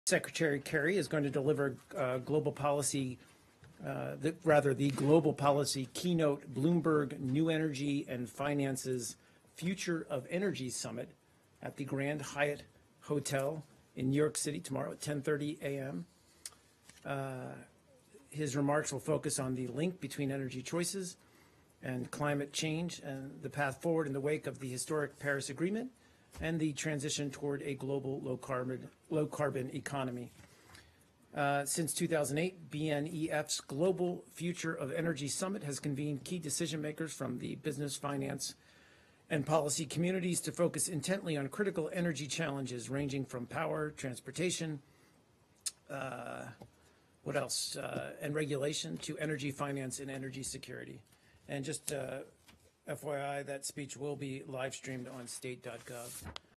Secretary Kerry to Deliver Keynote at Future of Energy Summit
Deputy Spokesperson Mark Toner leads the Daily Press Briefing at the Department of State on April 4, 2016.  on Secretary Kerry delivering a speech at Bloomberg's New Energy Finance Summit is available